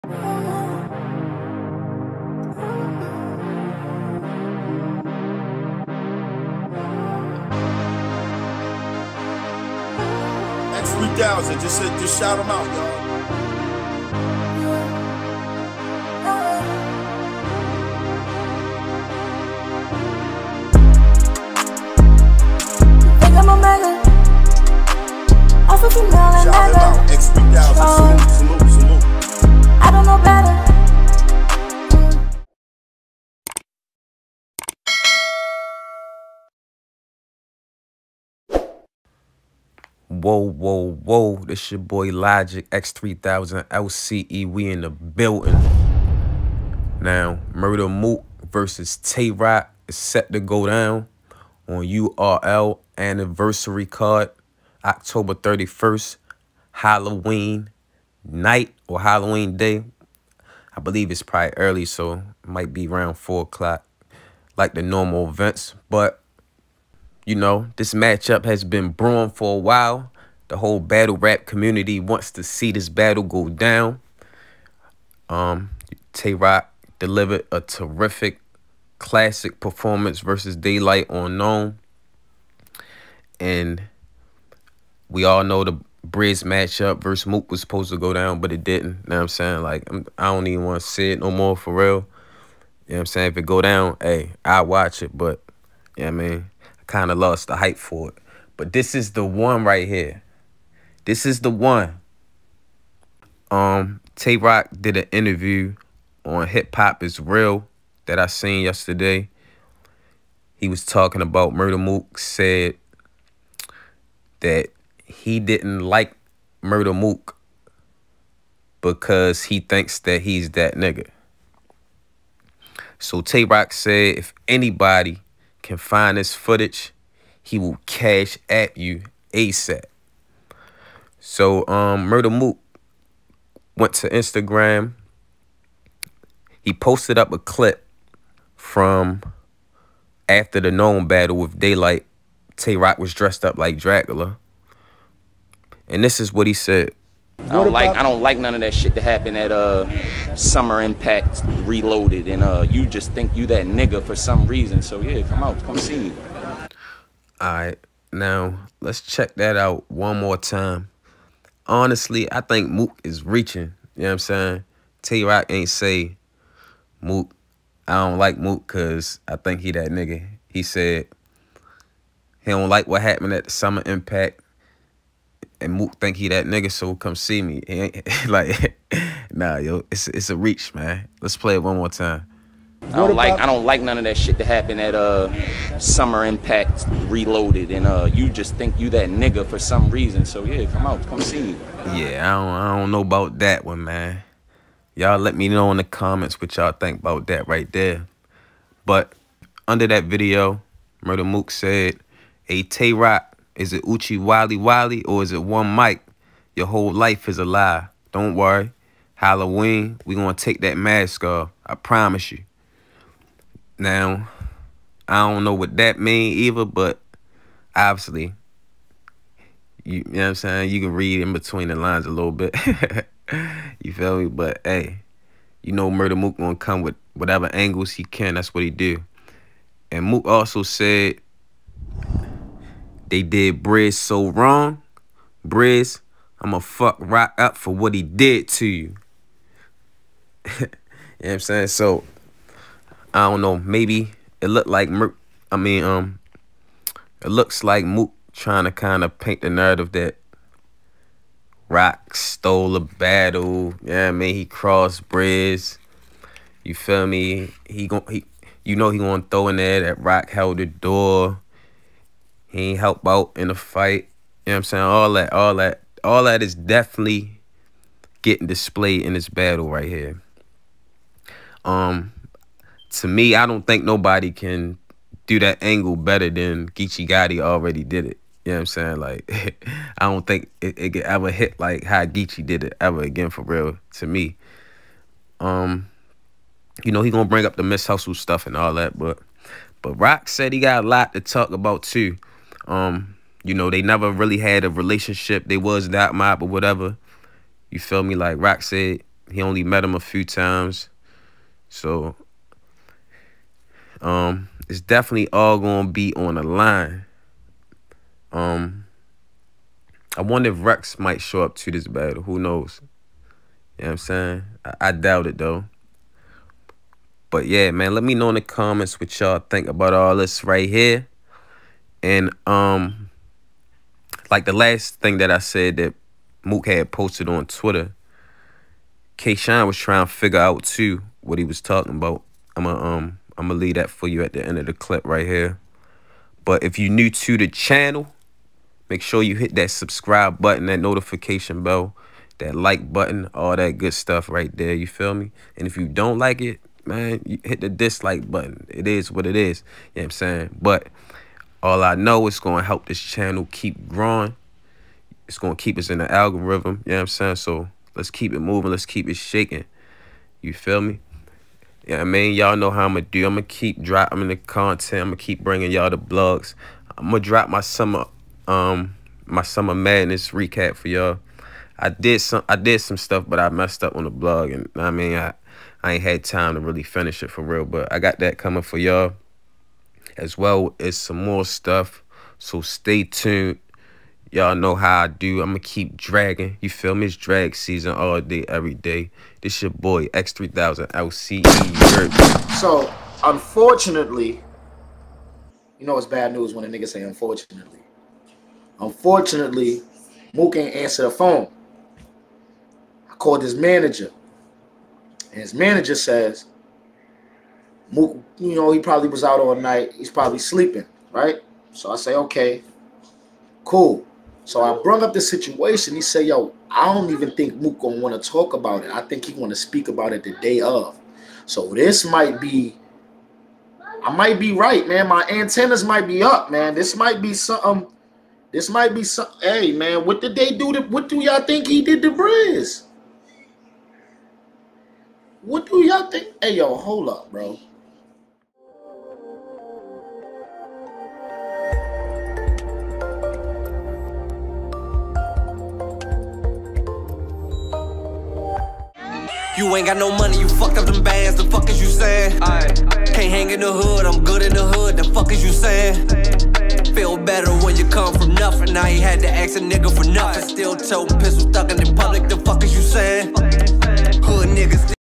MURDA MOOK VS TAY ROC IS HEATING UP! MOOK WANTS DAT CASHAPP CLAIMS HE FOUND THE VIDEO
Cash app: $LOGIKLCE